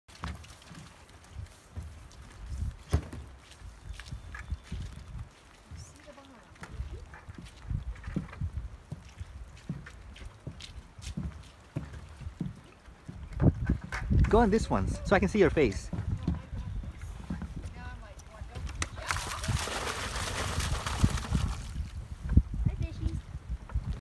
Jim Creek Stocks Twin Lakes 2017
Jim Creek's Management Intern makes the inaugural dump of 2017's first rainbow trout stocking at Jim Creek's Twin Lakes. Boat, canoe, and kayak rentals are currently available on Saturdays only until peak season operations begin in May, however Twin Lakes is open 7 days a week for day use and shore fishing. Questions?